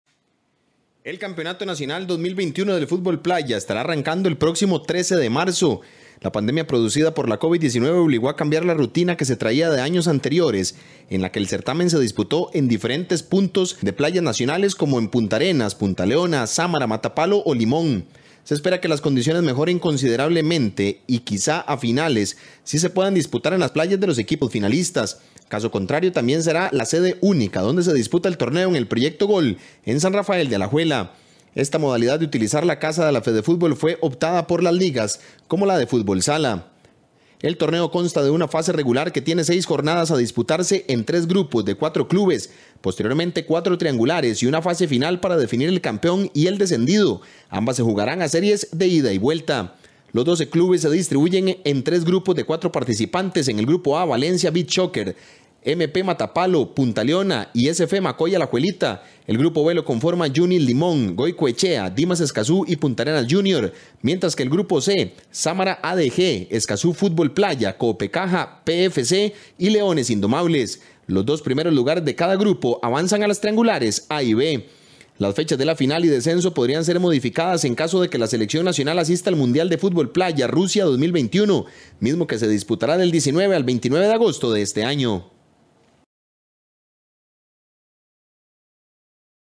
A pocos días de regresar el Fútbol Playa, este 13 marzo 2021 con 12 clubes en competencia
EL TORNEO DE FUTBOL PLAYA 2021 COMENZARÁ A PARTIR DEL 13 DE MARZO Y ENTRE SUS PARTICULARIDADES ESTÁ QUE SE JUGARÁ EN UNA SOLA SEDE Y CON 12 CLUBES EN COMPETENCIA.